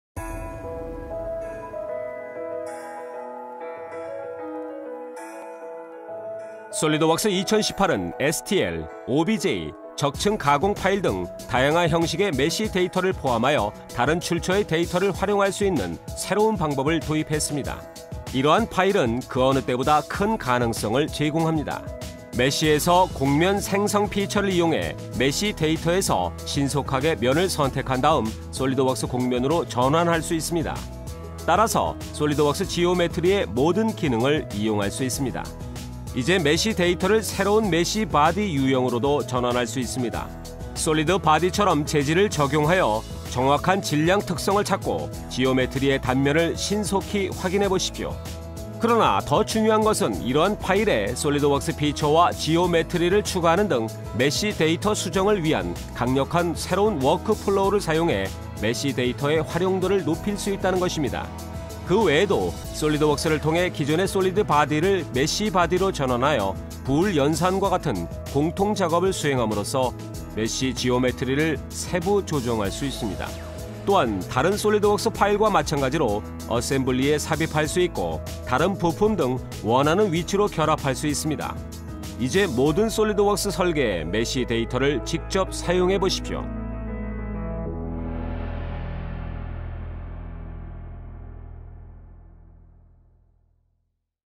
메시 데이터로 손쉽게 직접 작업 | 솔리드웍스 2018 신기능 | 메이븐 (MAVEN)
SOLIDWORKS 2018 - 메시 데이터로 손쉽게 직접 작업.  메시 데이터를 곡면 및 바디 지오메트리처럼 사용하세요. 메시에서 직접 곡면을 만들어 3D 메시 지오메트리를 네이티브 SOLIDWORKS 곡면으로 빠르게 변환하세요.

※솔리드웍스 공인 리셀러 ㈜메이븐